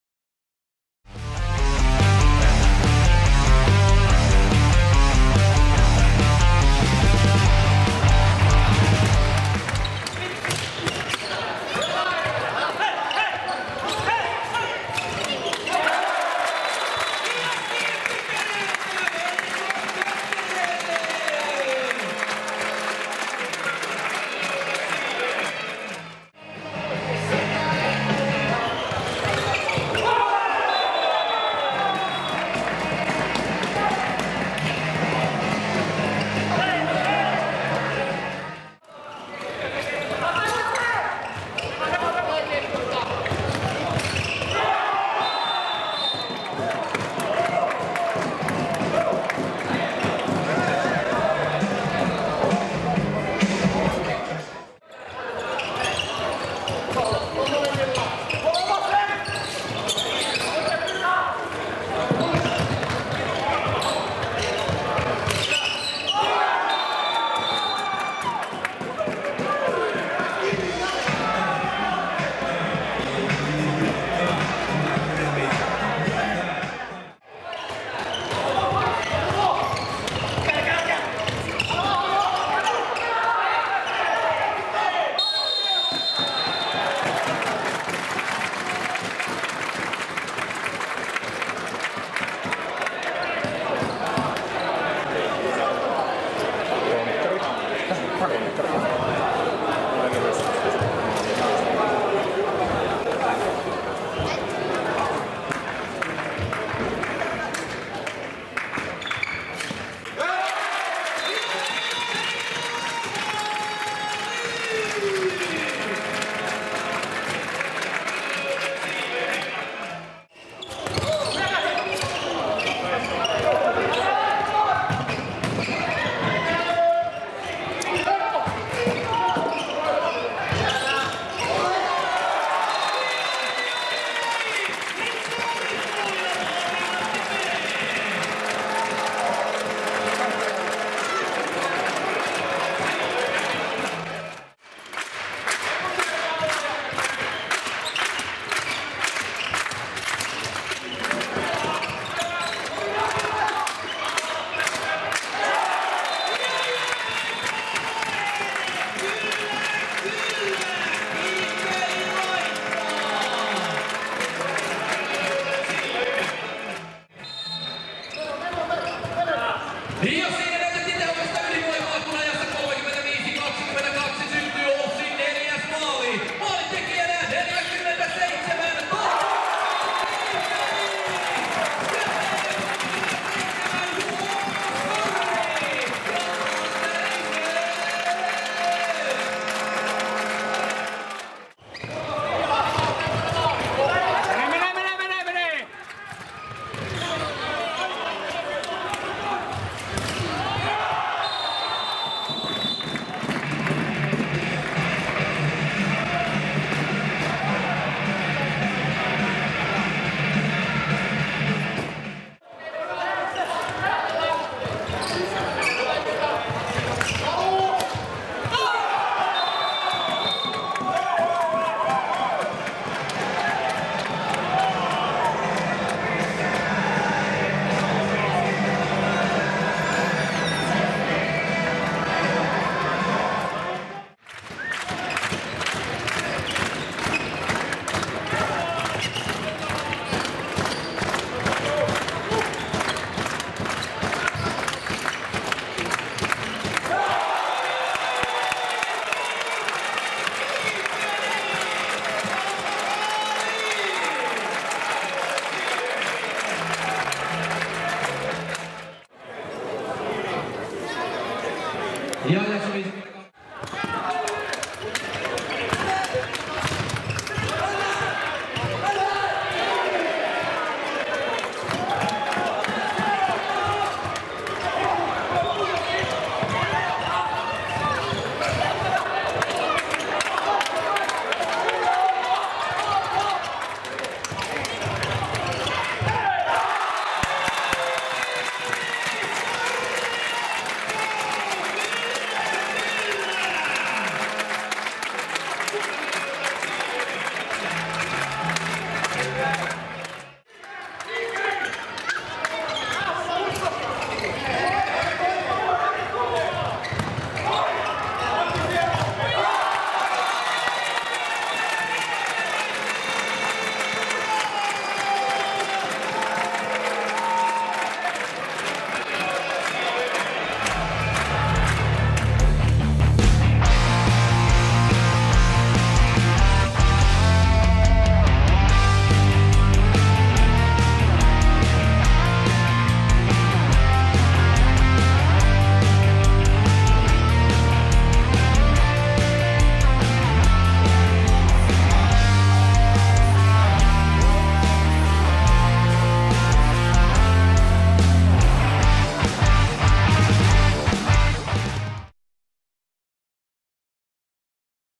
Maalikooste: OLS vs. M-Team (Su 12.02.2017)
Maalikooste Miesten Salibandyliigan ottelusta OLS (Oulu) vs. M-Team (Helsinki) ... Su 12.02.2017 @ Kastellin Monitoimitalo, Oulu ... Kuvaus & Editointi: Ari Holopainen, Musiikki: Erkki Pasanen